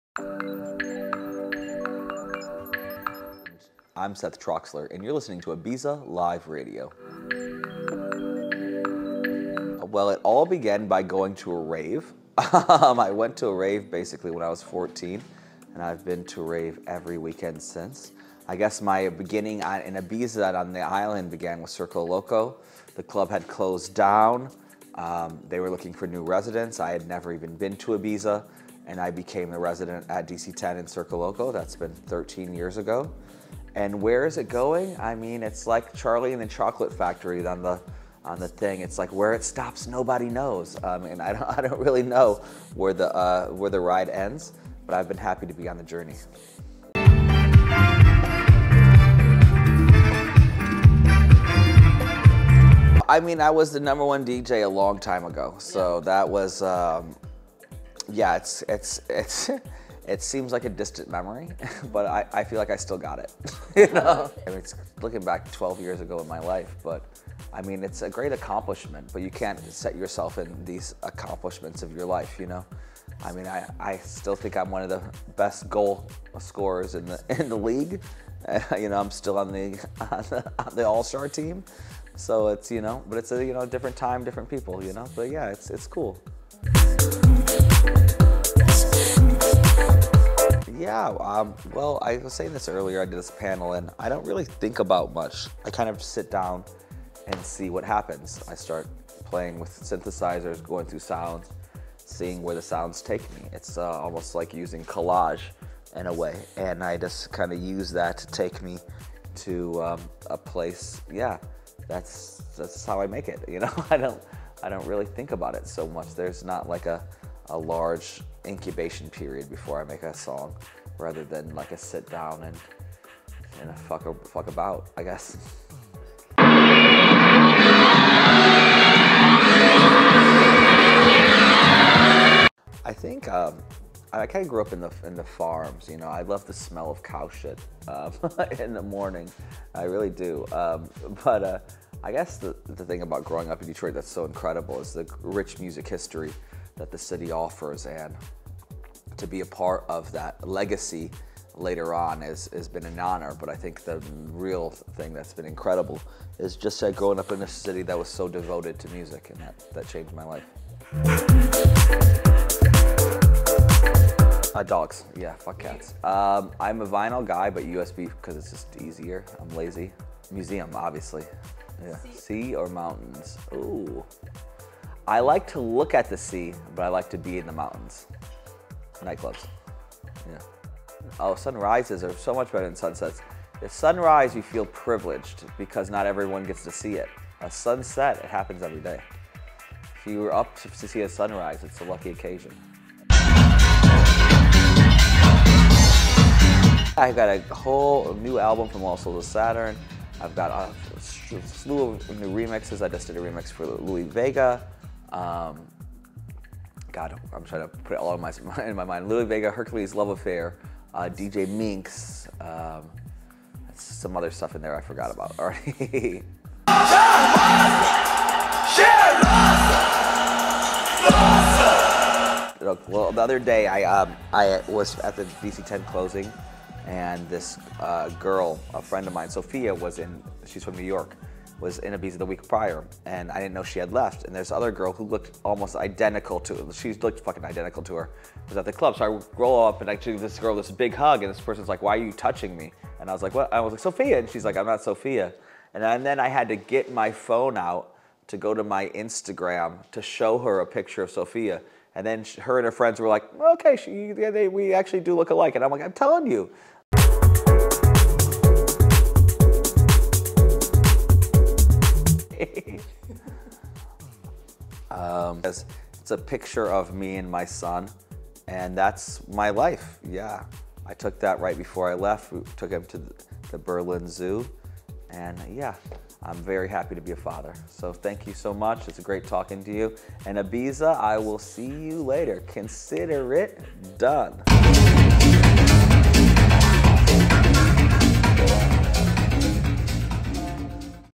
IBIZA LIVE RADIO - ADE – TALK WITH SETH TROXLER
Consider it done!

Yeap! We did it! Short and sweet conversation with amazing Seth Troxler ,an icon on Ibiza’s club scene for more than a decade he has been delivering sets that made us dance from dusk till dawn…

Many thanks to The Media Nanny for organizing everything impeccably, and for friendly welcoming everyone at beautiful hotel De L’Europe Amsterdam
during the overwhelming Amsterdam Dance Event.

Seth Troxler is one of electronic music’s most instantly recognisable characters. He has carved out his own unique path as a cultural commentator, art curator, restaurateur and entrepreneur.

Descended from a family of cowboys and Baptist preachers, he’s part African-American, with a singular approach to viewing the world through the prism of contemporary music culture.